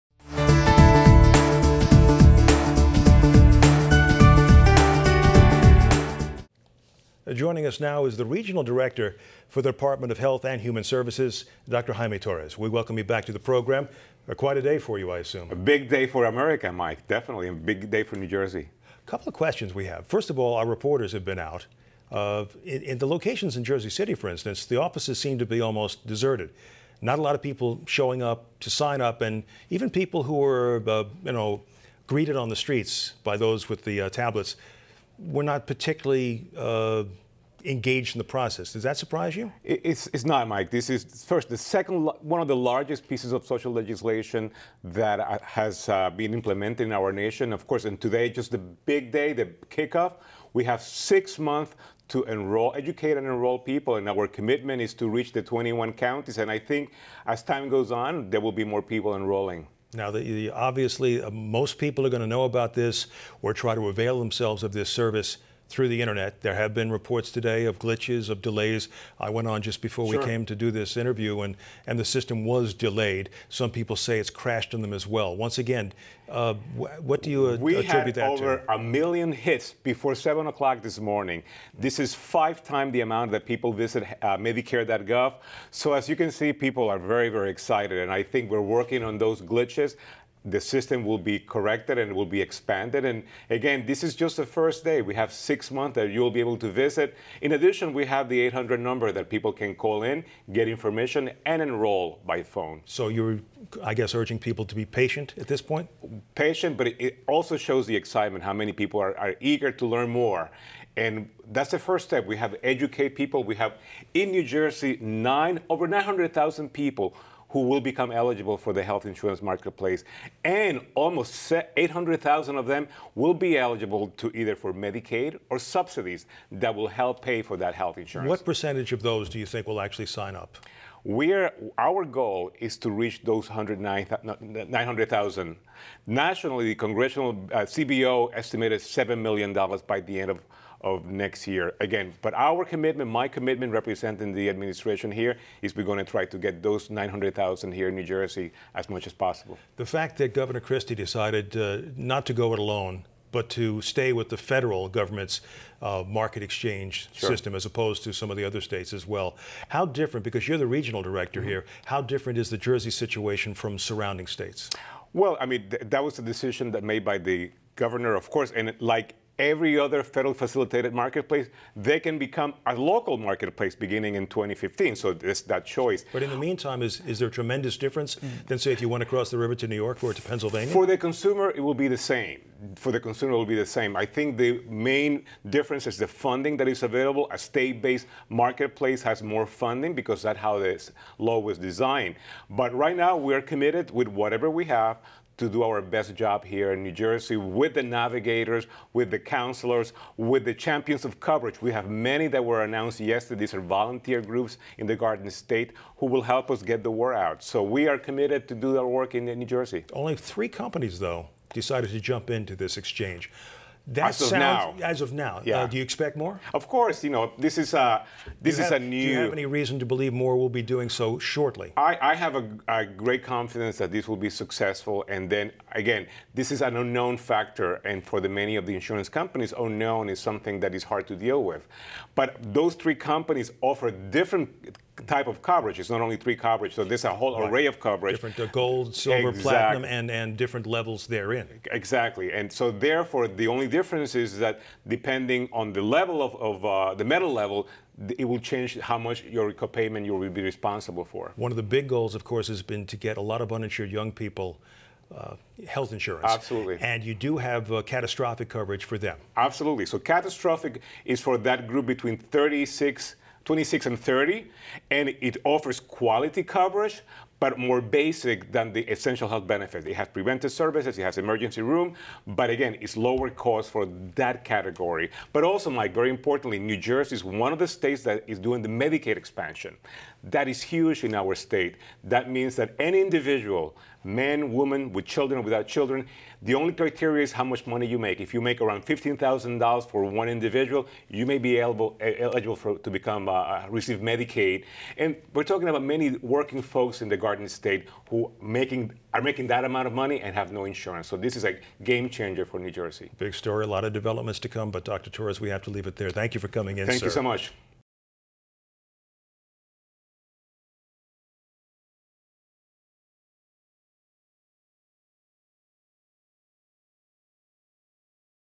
Department of Health Director Says Start of Health Exchange Enrollment 'Big Day for America'
Dr. Jaime Torres, regional director of the U.S. Department of Health and Human Services, says his goal is to reach all 900,000 New Jerseyans who are eligible to enroll in the health exchanges through the Affordable Care Act. For more New Jersey news, visit NJ Today